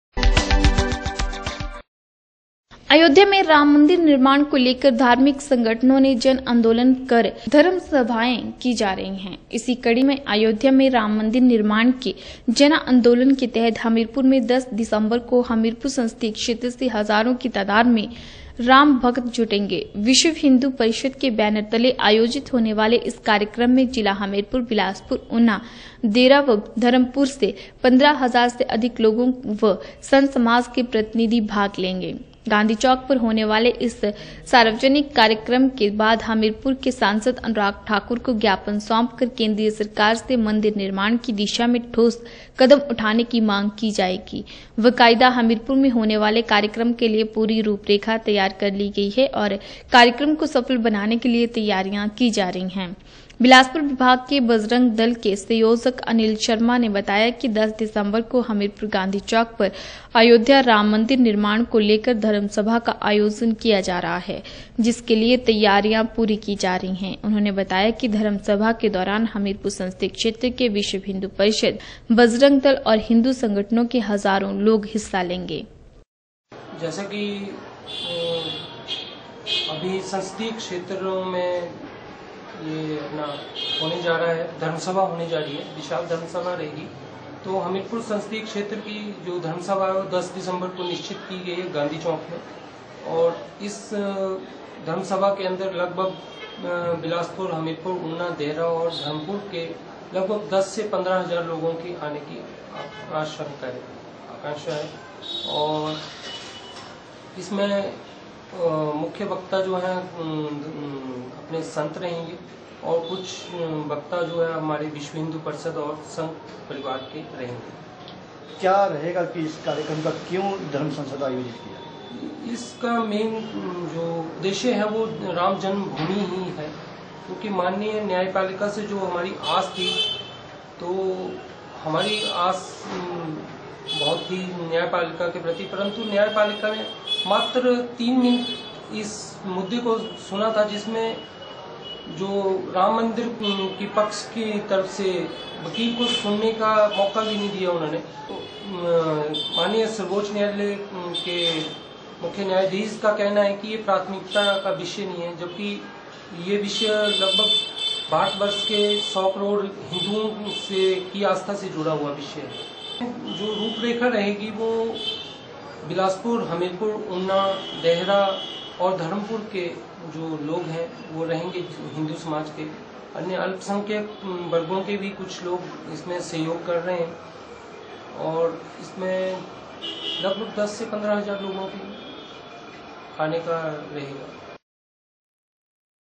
अयोध्या में राम मंदिर निर्माण को लेकर धार्मिक संगठनों ने जनआंदोलन कर धर्मसभाएं की जा रही
अयोध्या में  राम  मंदिर निर्माण को लेकर धार्मिक संगठनों ने जनआंदोलन कर धर्मसभाएं की जा रही है । इसी कडी में अयोध्या में राम मंदिर निर्माण के जनांदोलन के तहत हमीरपुर में 10 दिसंबर को हमीरपुर संसदीय क्षेत्र से हजारों की तादाद में राम भक्त जुटेंगे। विश्व हिंदू परिषद् के बैनर तले आयोजित होने वाले इस कार्यक्रम में जिला हमीरपुर, बिलासपुर, ऊना, देहरा व धर्मपुर से 15 हजार से अधिक लोगों व संत समाज के प्रतिनिधि भाग लेंगे।